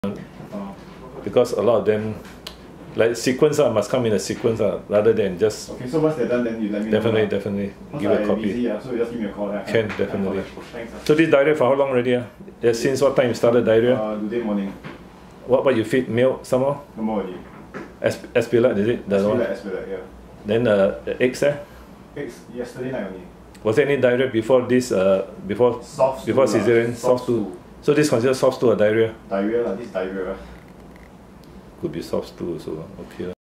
Poodle Caesarean section 2 days later - diarrhoea
Best not to introduce new food after birth as it may lead to diarrhoea